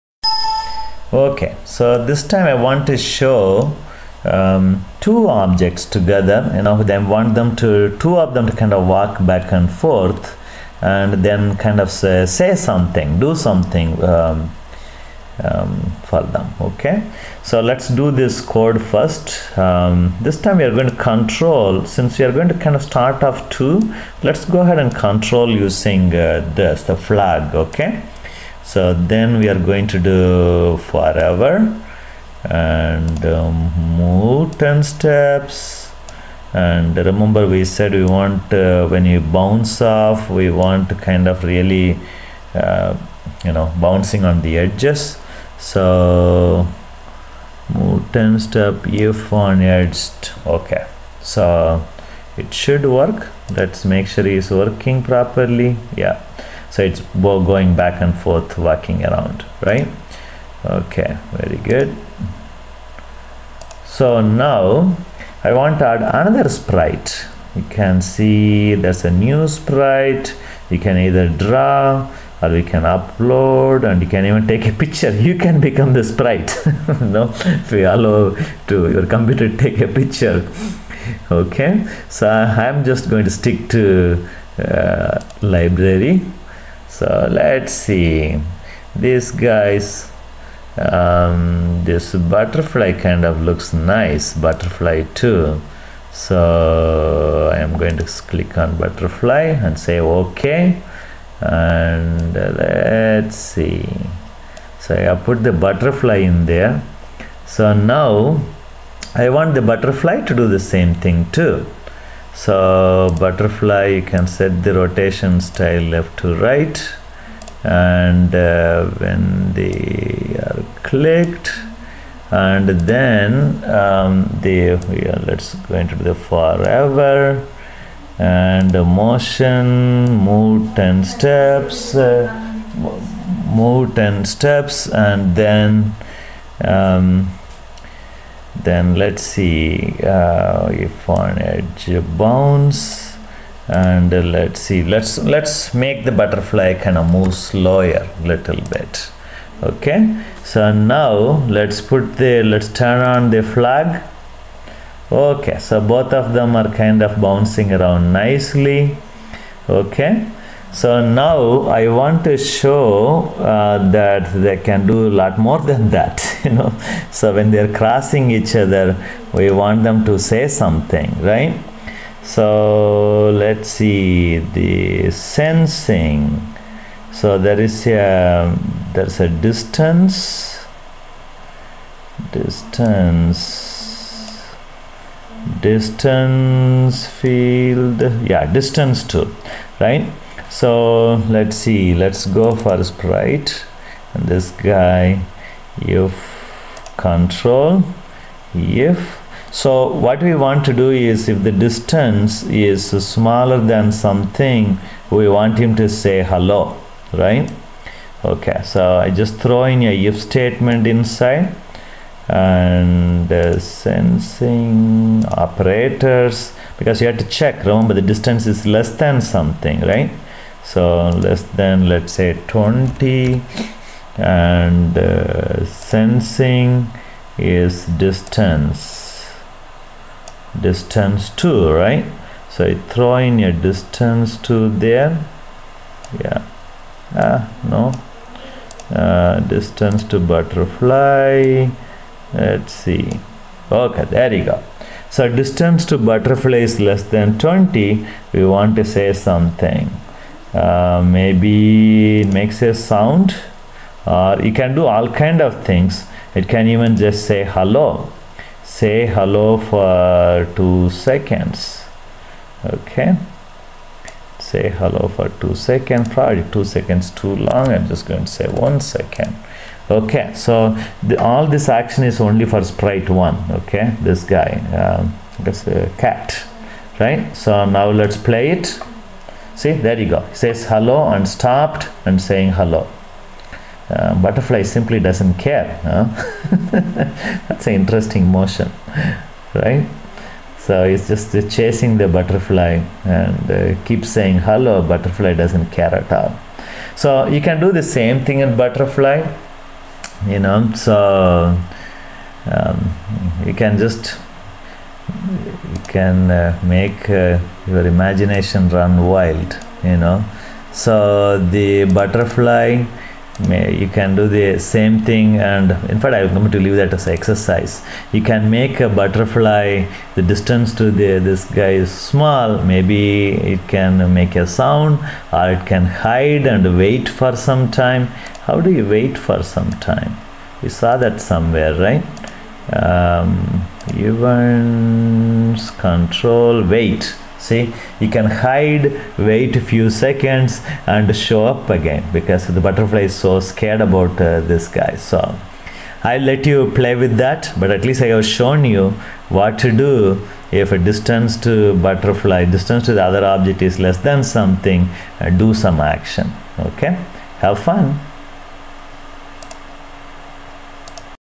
Scratch : 2 objects moving back and forth
Scratch programming - 2 objects moving back and forth. When they cross each other, they can do some action too.